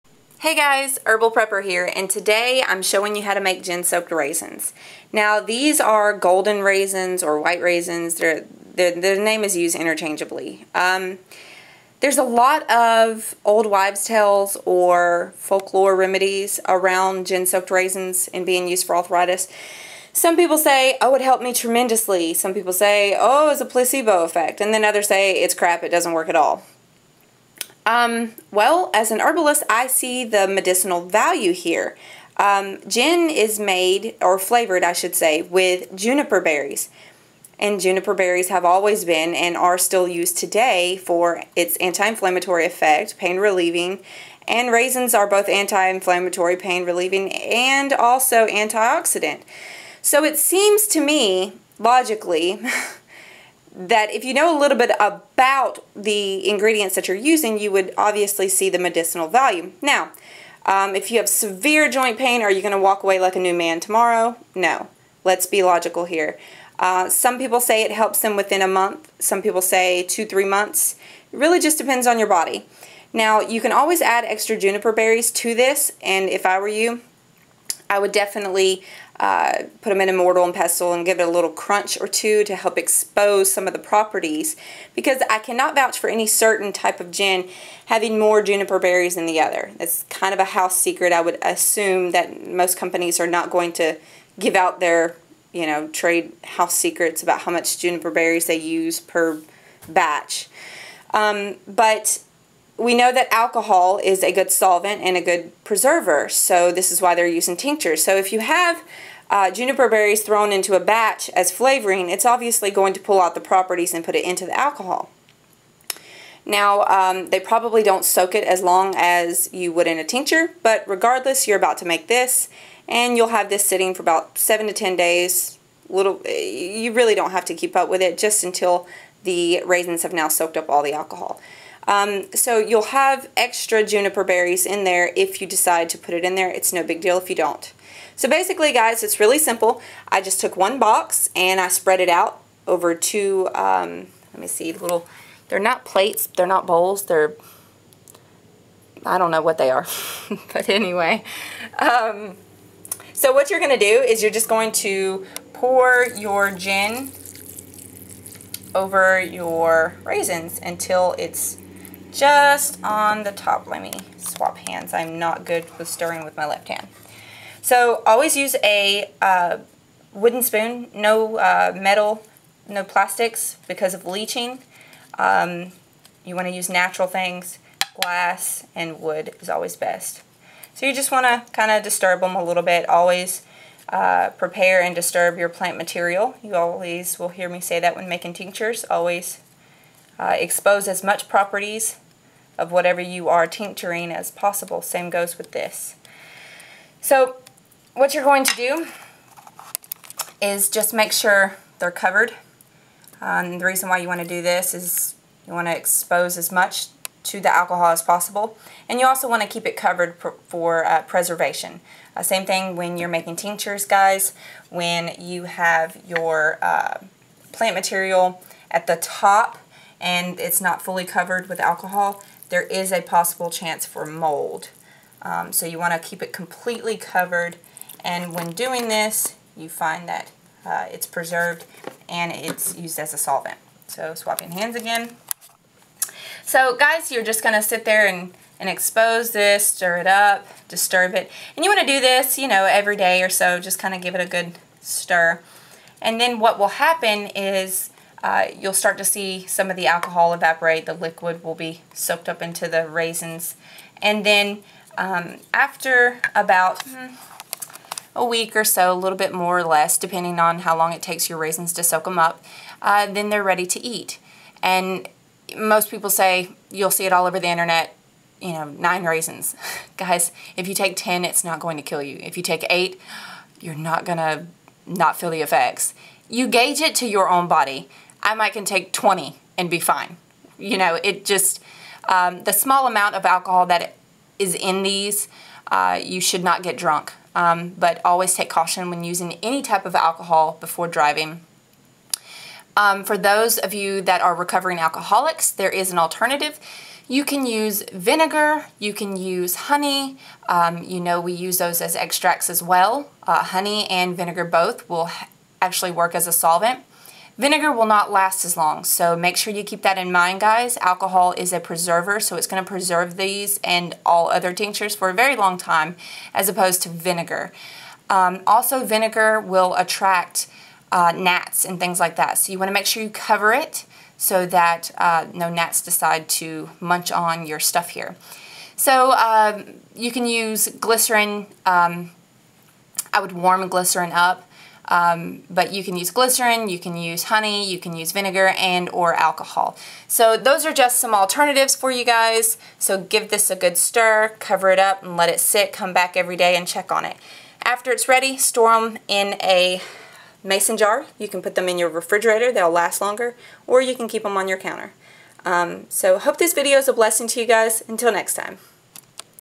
How to make gin soaked raisins
Gin soaked raisins are used for arthritis and are known to be an old wives tale but has medicinal value due to the juniper berries and the raisins. Try this along with your other home remedies and see if you have any improvement!